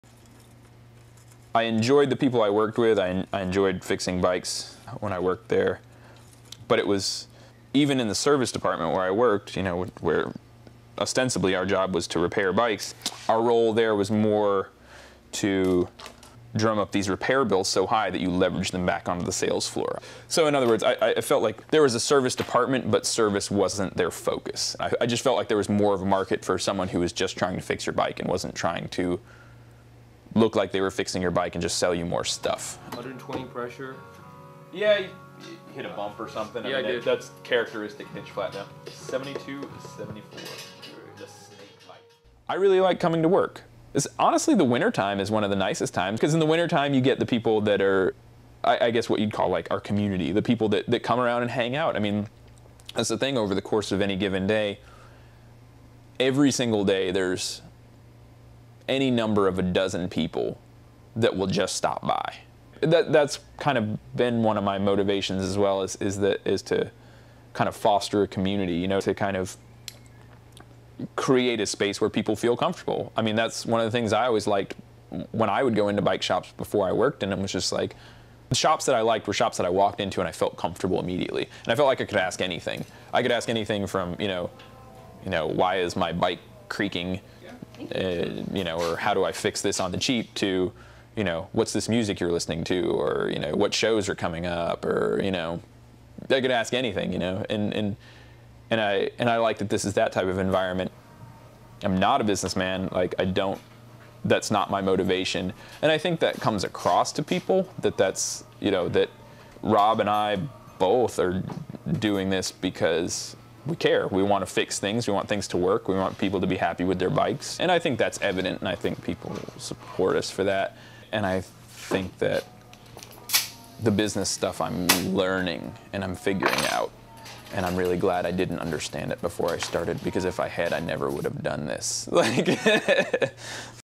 A Better Bike Shop
Jason Merrill, owner of Back Alley Bikes in Carrboro, NC, talks about why he started his business.